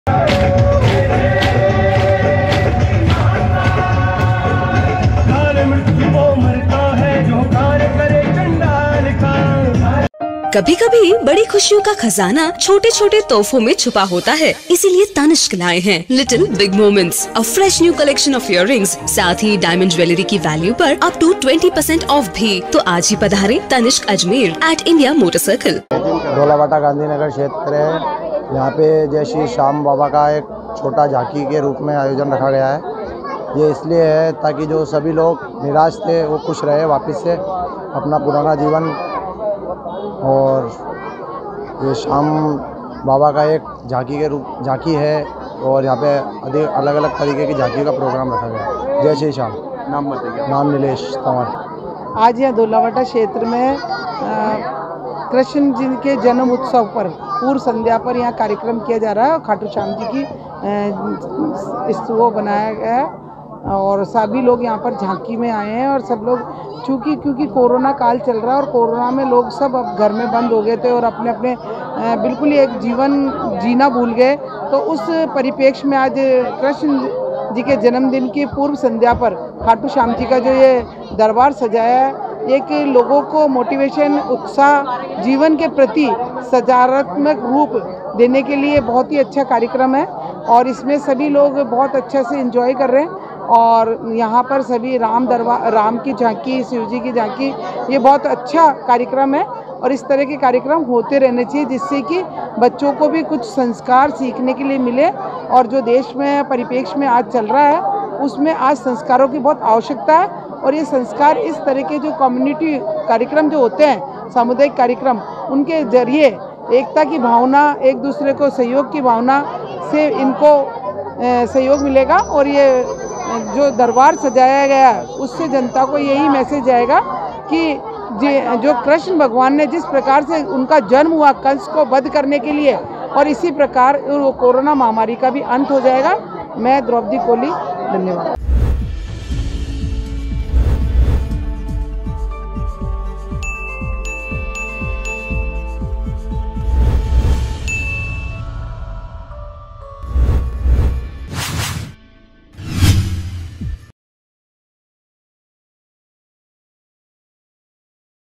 Ajmer | धोला भाटा क्षेत्र में कृष्ण जन्माष्टमी की पूर्व संध्या पर सजी झांकियां| MTTV INDIA
धोला भाटा क्षेत्र में कृष्ण जन्माष्टमी की पूर्व संध्या पर सजी झांकियां
झांकियां देखने शहर भर से लोगों की उमड़ी भीड़
पिछले 2 वर्षों से कोरोना की वजह से ठप पड़ी जिंदगी को जन्माष्टमी पर संजीवनी